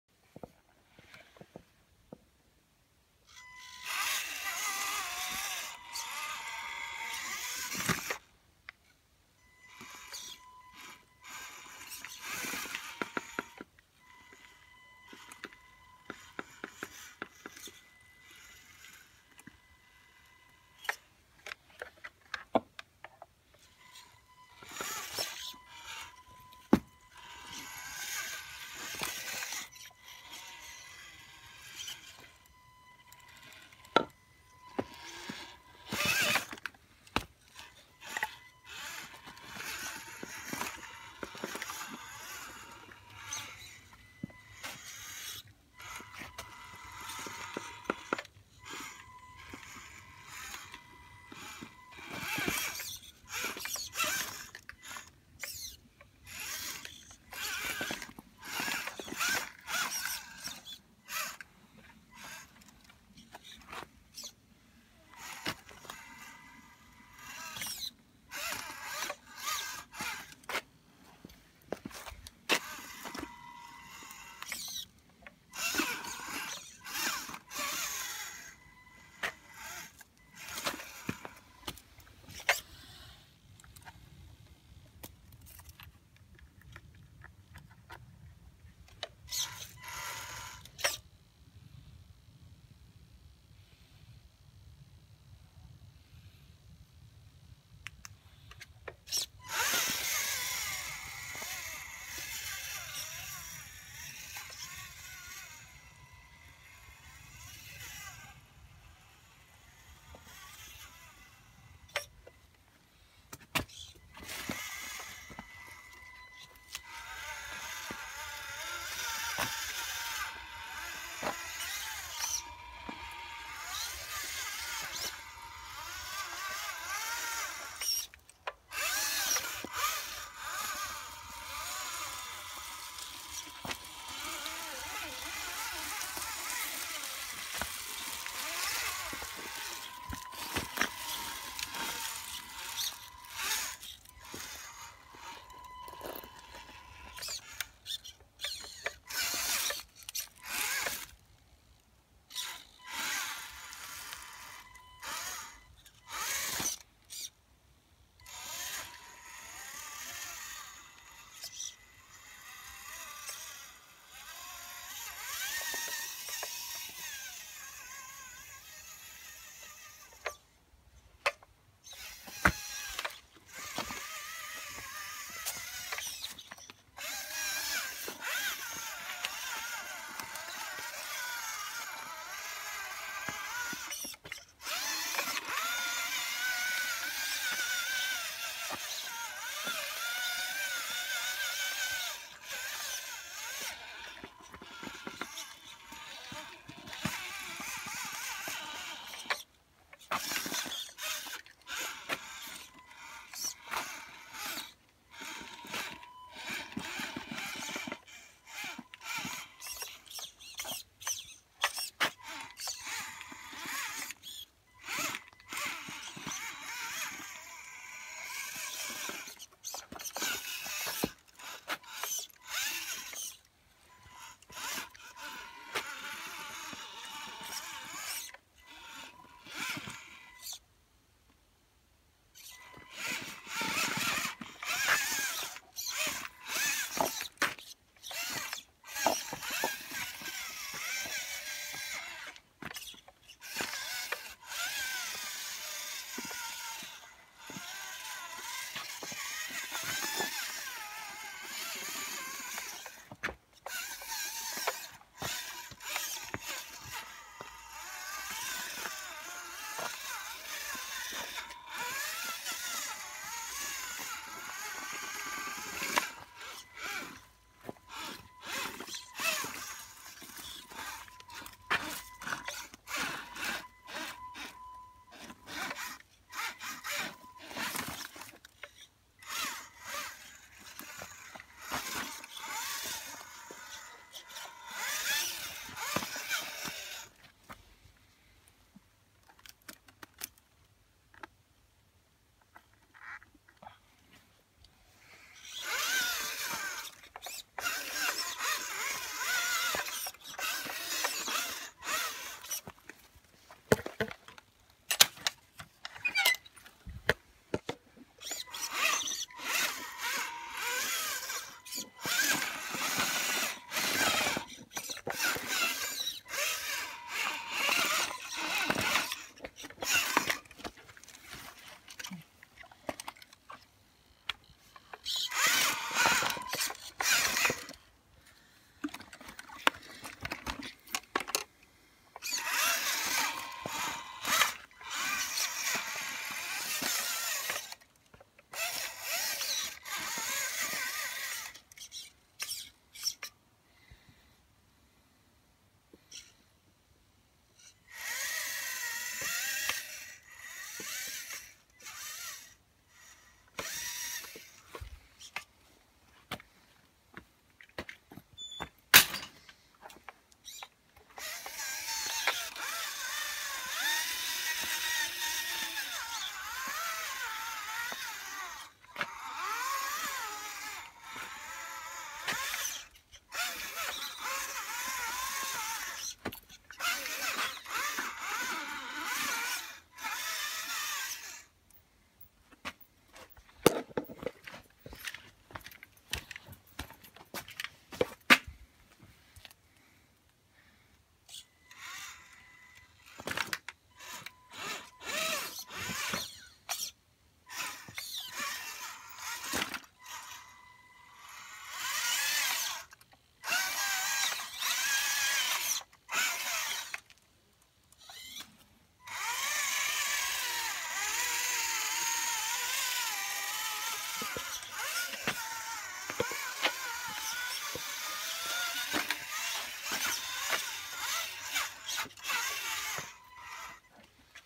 Scale run at night
Just testing the rig in the dark now I removed the coils on the shocks.